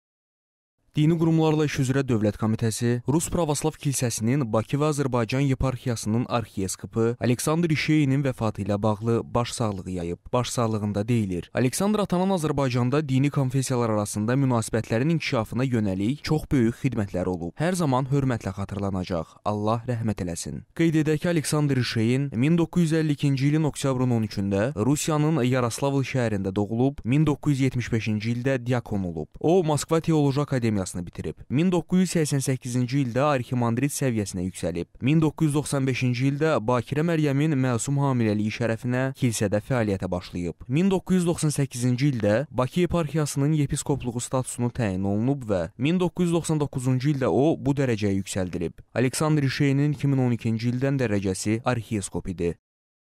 Aleksandr İşein vəfat edib
DQİDK: Aleksandr atanın çox böyük xidmətləri olub